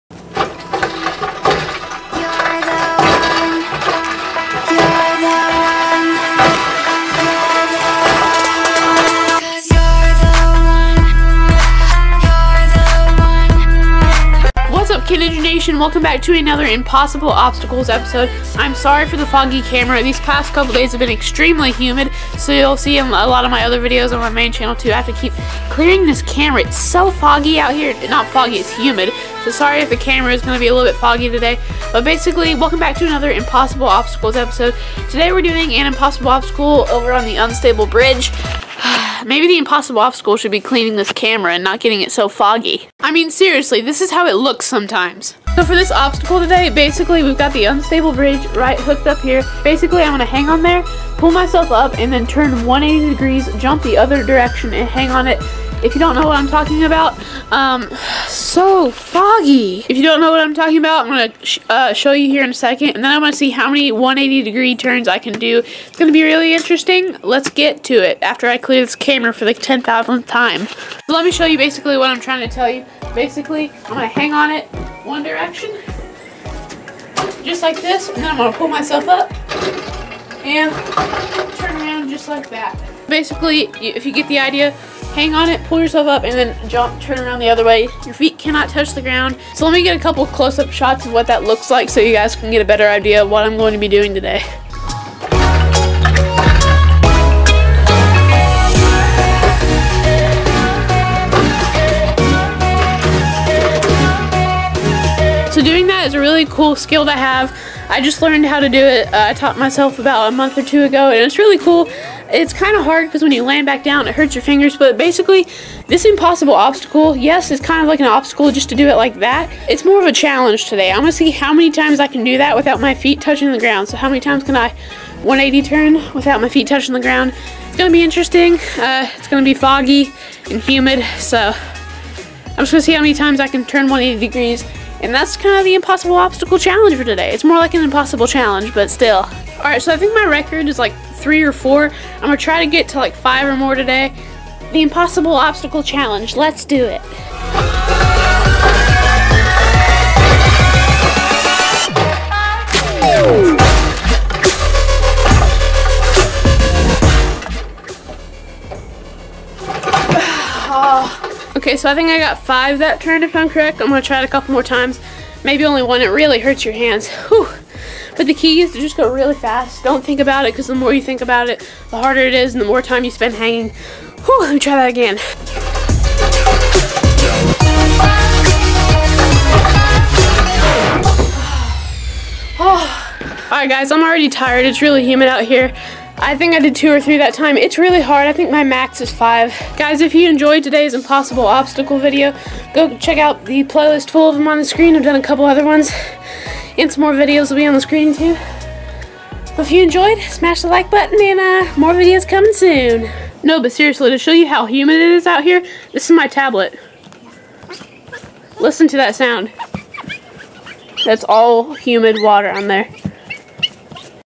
Unstable Bridge 180° Hops | Impossible Obstacles Ep. 3
Today I see how many times I can jump/turn 180° on my unstable bridge!

If you want to know how to get any specific song that I used, just let me know and I'll be happy to help.